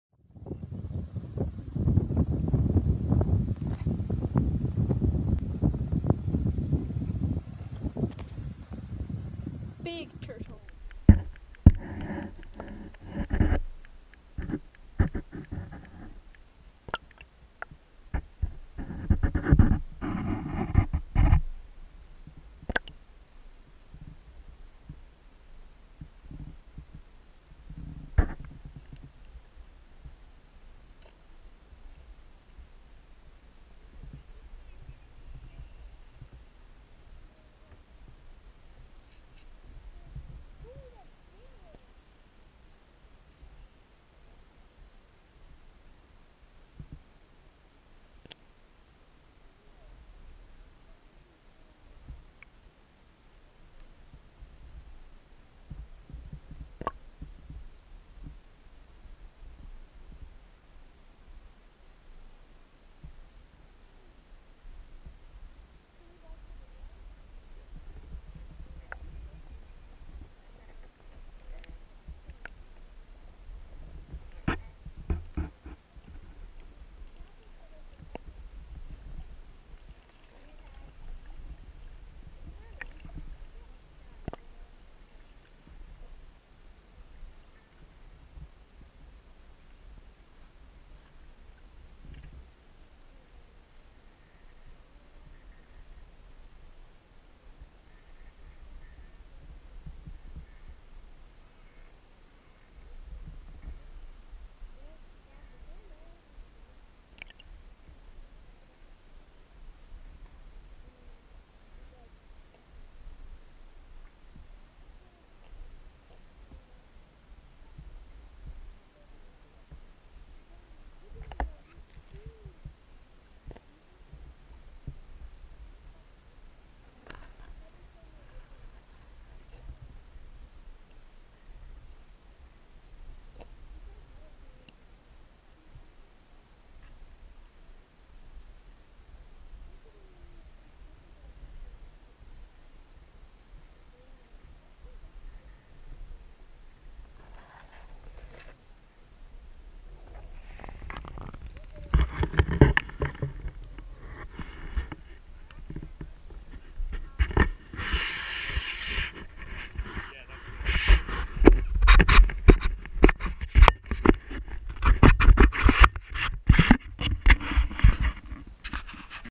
Ryleigh Feeding Worms To Santa Claws The Turtle
The Kid's named this guy - Santa Claw's